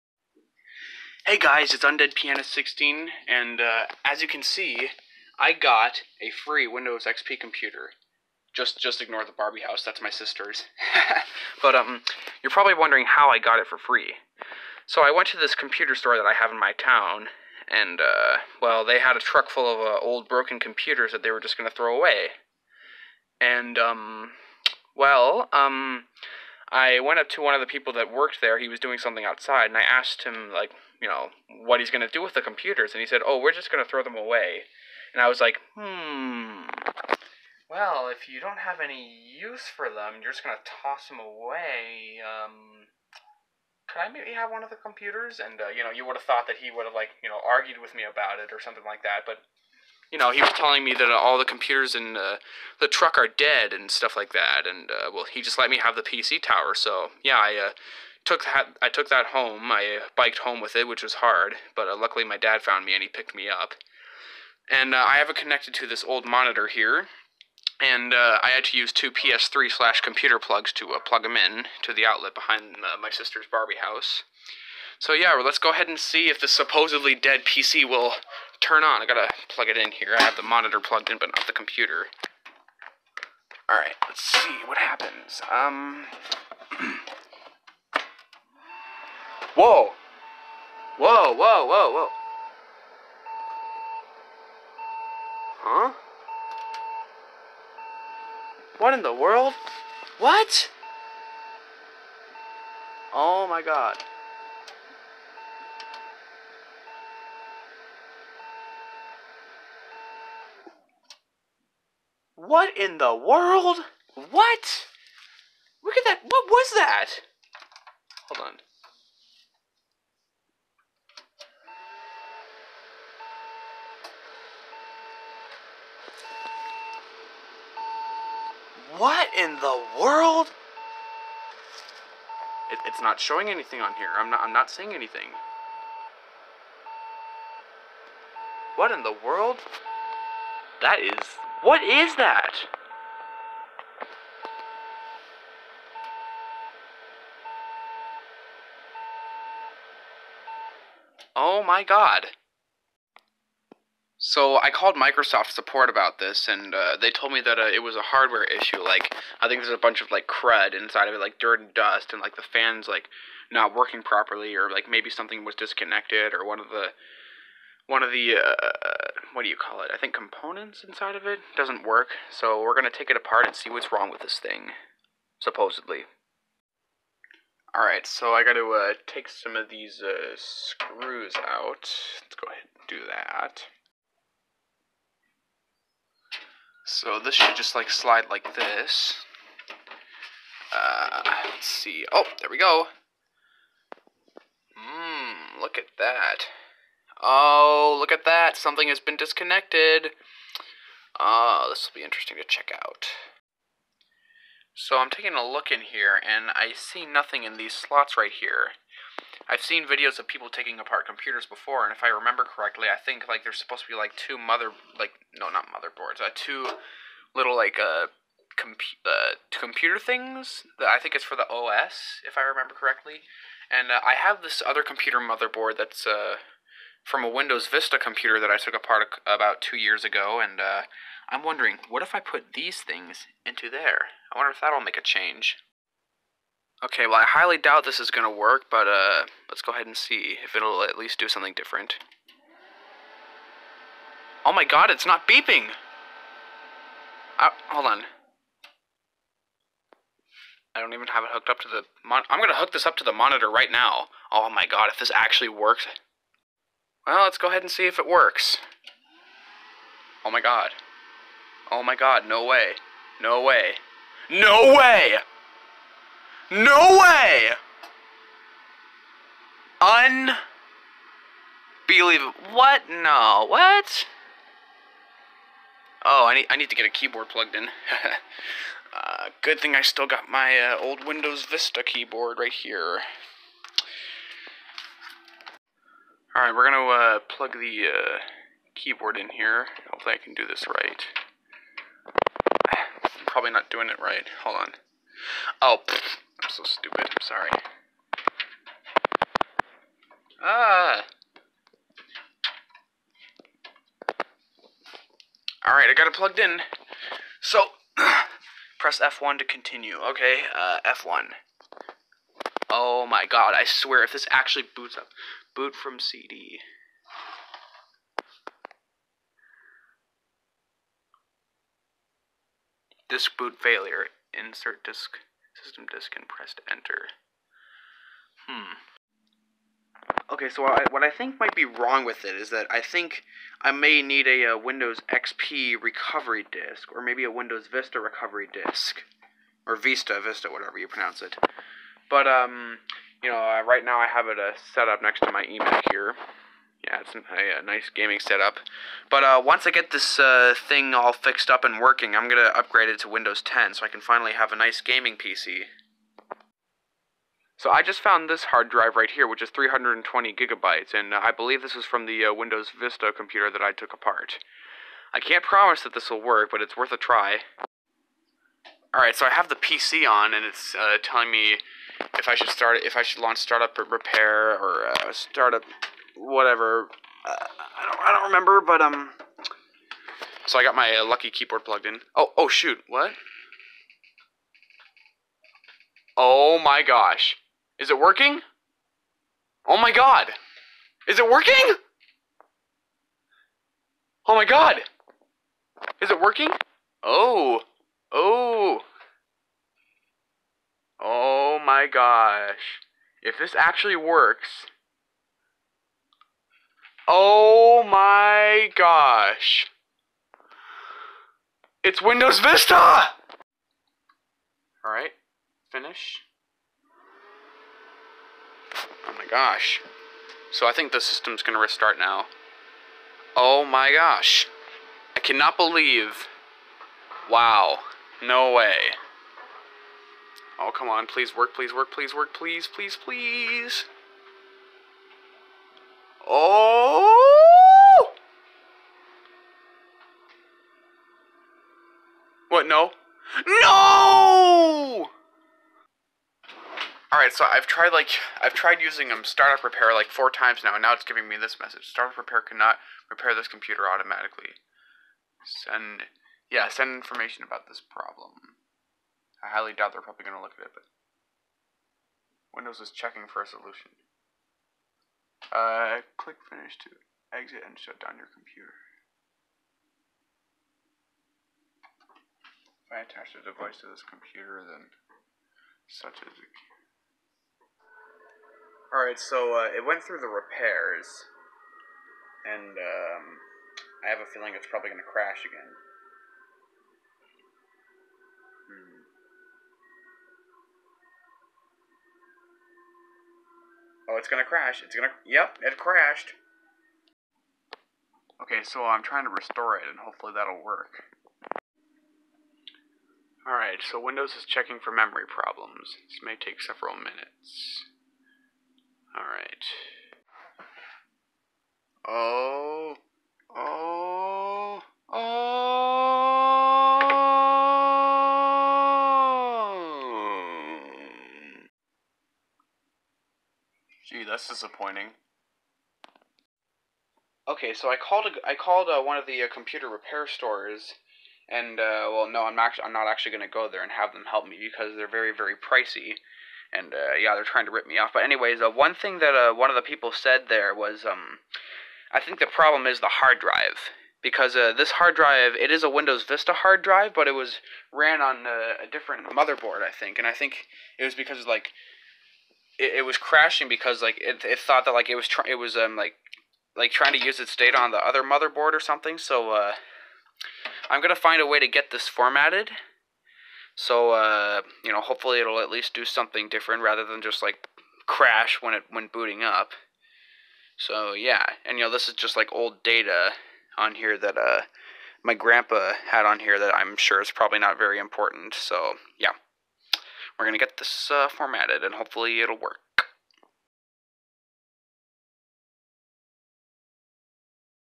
Attempting to Fix a Trashed Computer Part #1
Okay so maybe I'm not the best with computers lol. I know some of you guys are so hopefully you can please help me in some way. I was to lazy to add an intro and outro. Sorry...